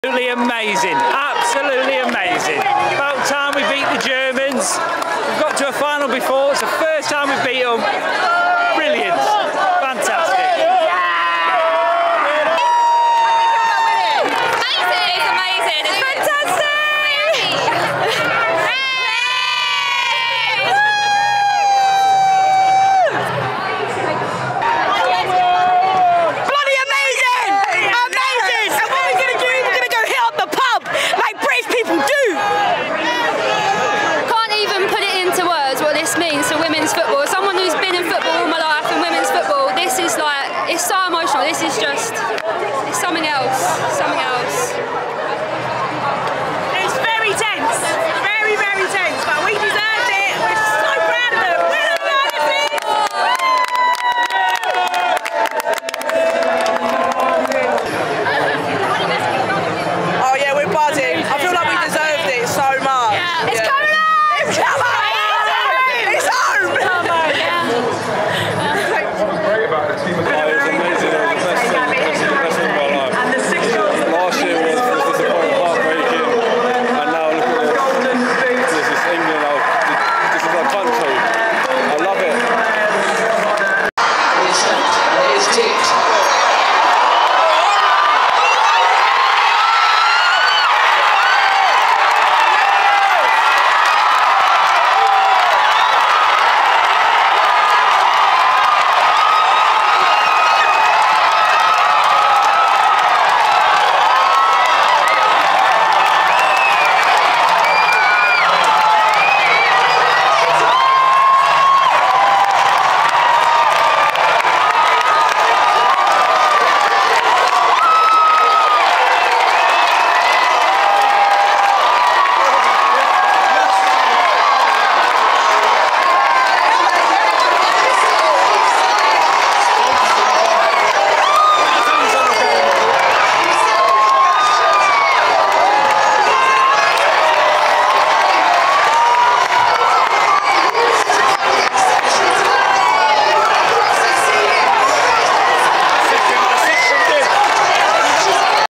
ENGLAND EURO 2022 WINNERS: The England Fans React to The Lionesses Beating Germany 2-1 in the Final
+ CONTENT:
* Fans at Trafalgar Square give their verdict on England's 2-1 win over Germany in the Euro 2022 final. Also scenes of the winning goal